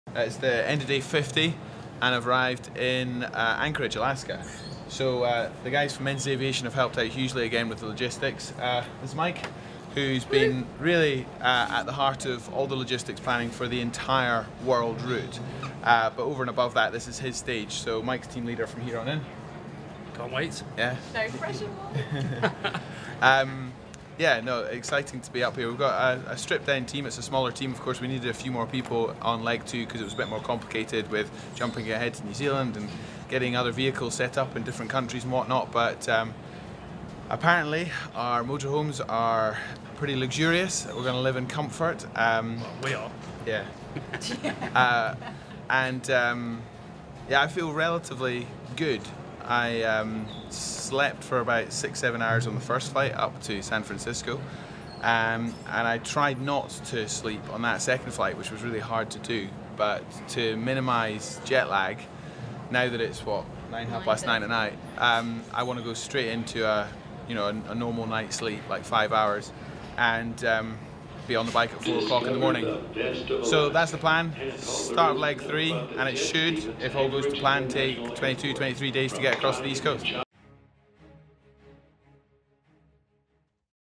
Artemis World Cycle: Arrival in Anchorage
Mark Beaumont is attempting to cycle around the world in 80 days. Having already powered across Europe, Asia & Australasia, Stage 3 sees him take on North America, from Anchorage to Halifax. For more information and to follow the adventure,